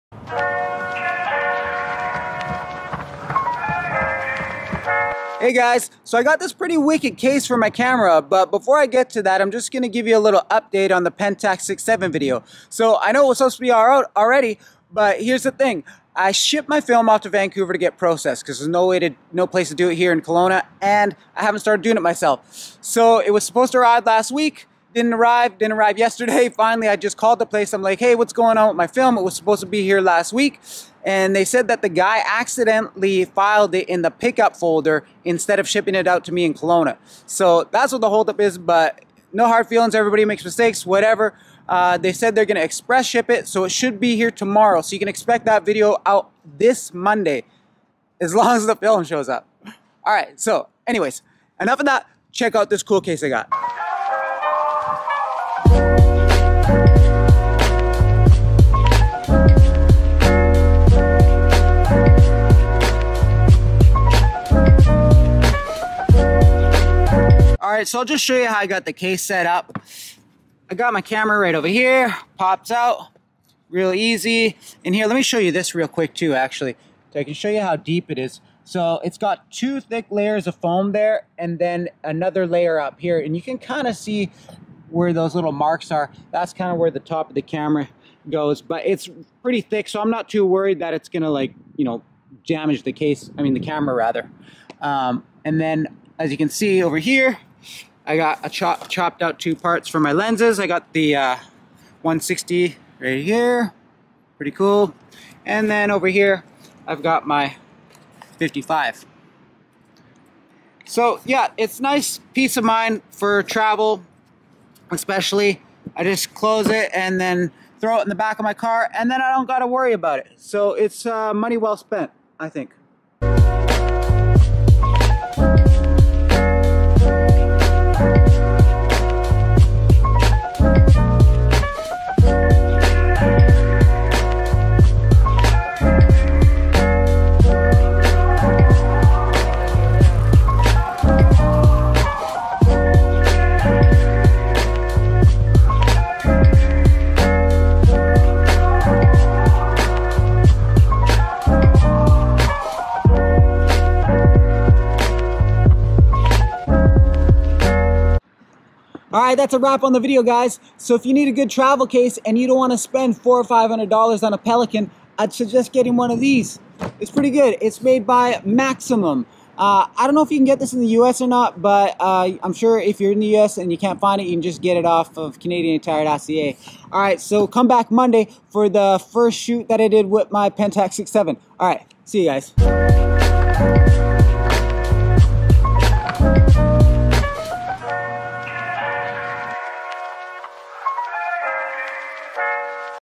Pelican Case Alternative for my Pentax 67
I bought this great alternative to a Pelican case for my Pentax 6x7 medium format film camera. It fits my Pentax 67 camera body with my 105mm f/2.4, 165mm f/2.8 & 55mm f/4 perfectly with some room to spare. The case is made my a company called Maximum and I bought it at store called Canadian Tire.

I think this is a great travel case for whatever photography gear you got. It's definitely a nice alternative to the Pelican 1650 and is at least half the price (I got mine on sale for about $150 Canadian). Nice piece of mind when you got loads of bags in a packed car or truck.

I filmed this video with a Sony zv-1 at Gyro Beach Park in Kelowna, BC Canada.